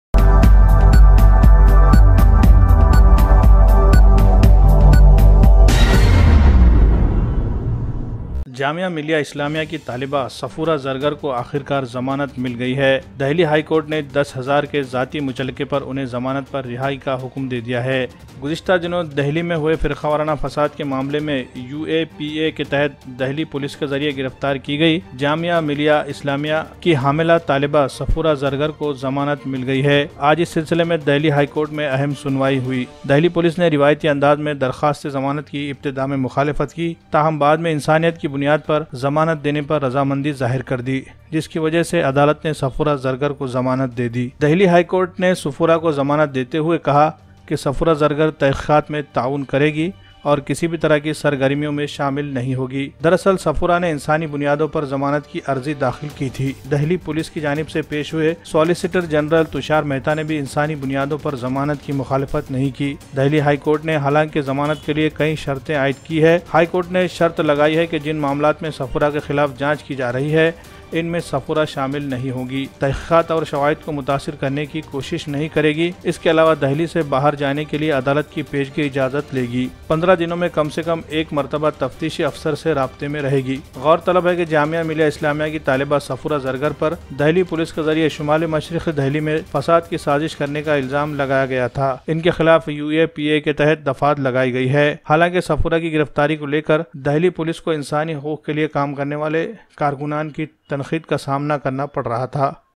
SafuraZargar जामिया मिलिया इस्लामिया की स्टूडेंट सफुरा ज़रगर को दिल्ली हाई कोर्ट से मली ज़मानत
jamiya miliya islamiya student safoora zargar get bail from delhi high  in today safroora zargar social activist and  famous anti CAA NRC protester